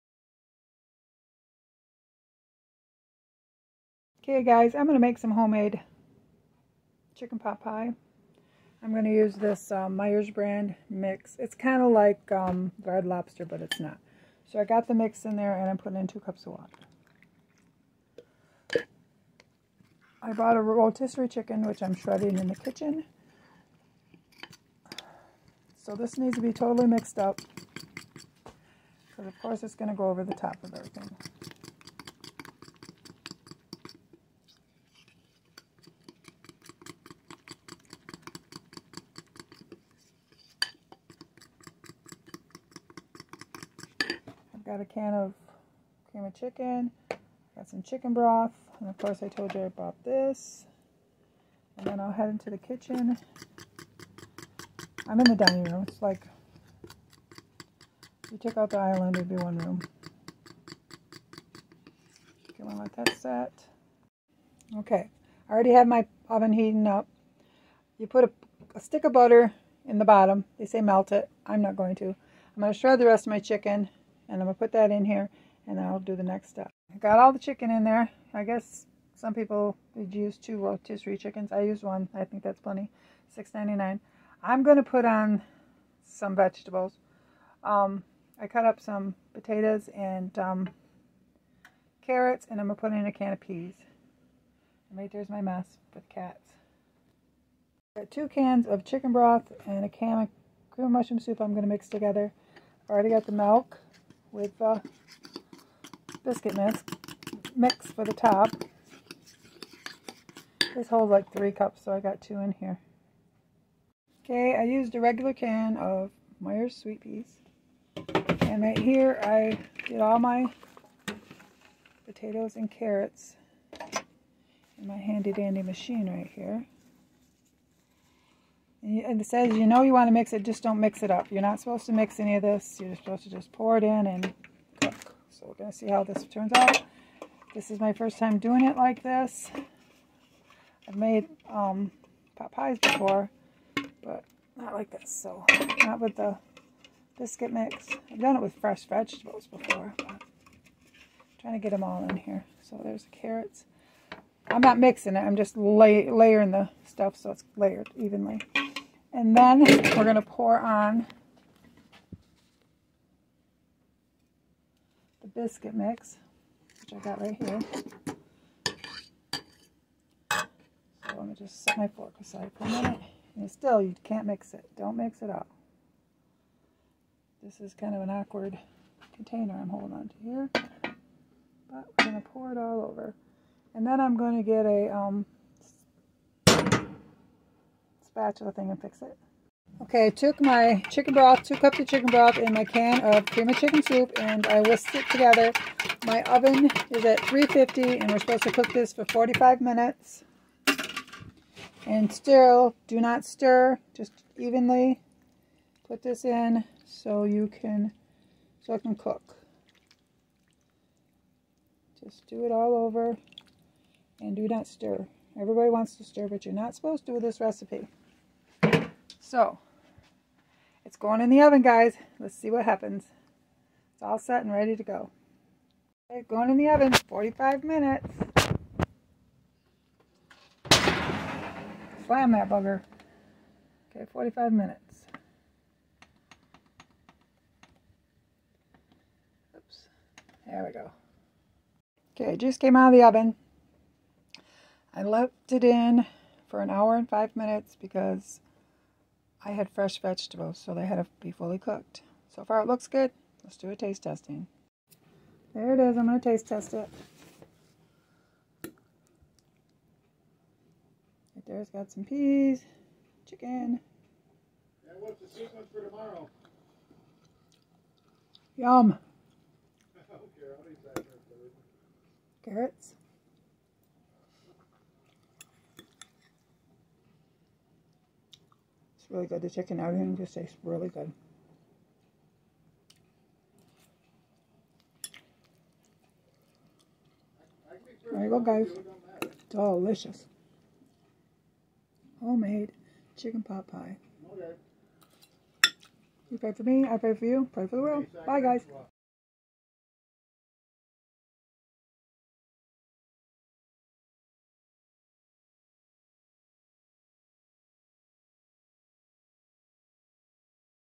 Homemade Chicken Pop Pie Recipe 😋 recipe or you can use leftover turkey leftovers or ham
You could make this with leftover turkey or ham